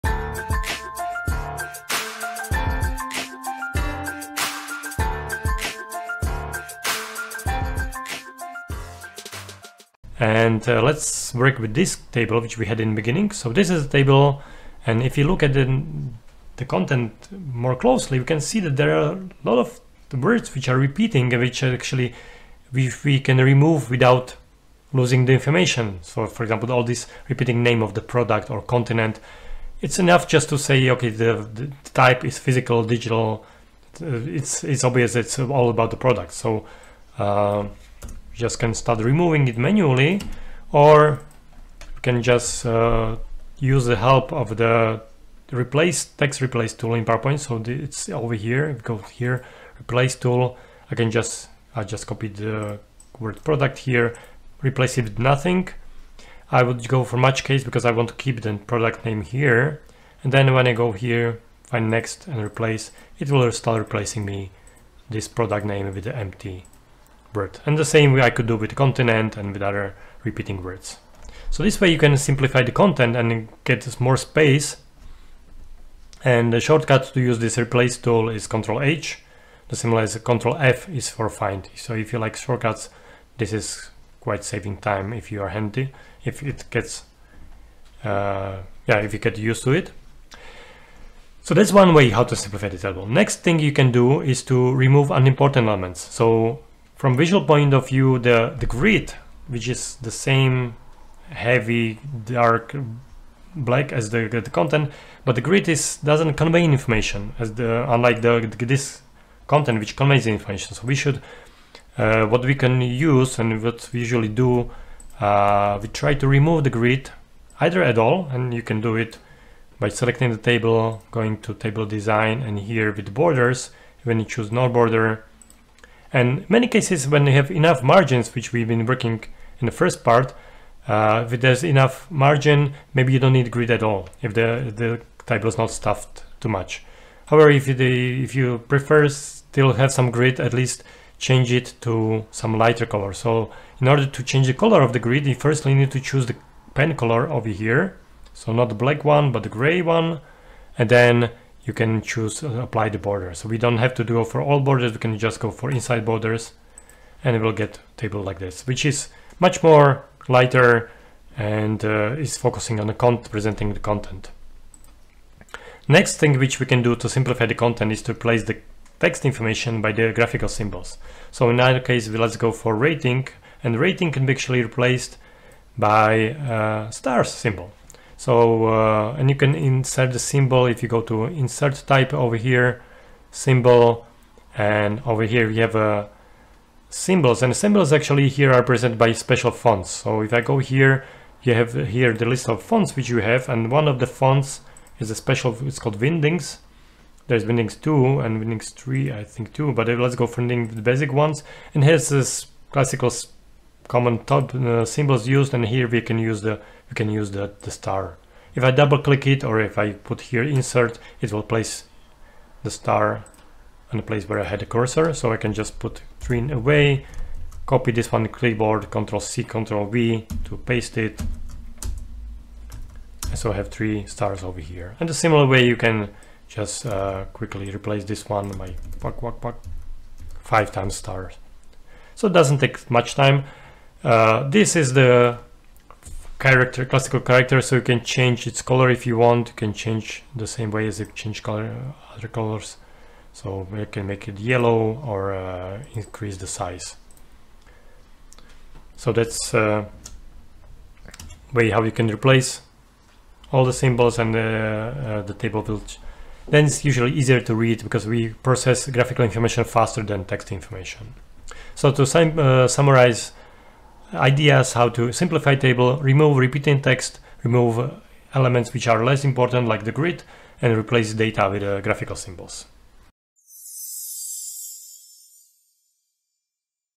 SIMPLIFY PowerPoint Table Quickly - PPT Tricks for Complex Structures
Learn how to simplify complex PPT tables using a few smart PowerPoint tricks and elevate your slides like a PRO.